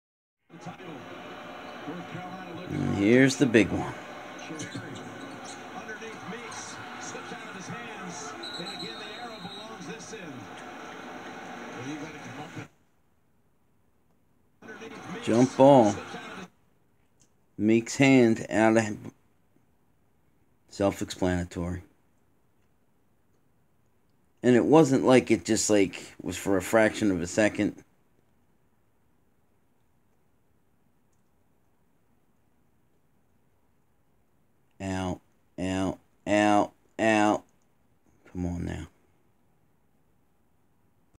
bad Calls- n carolina vs gonzaga championship game
one of  many calls that made it impossible for gonzaga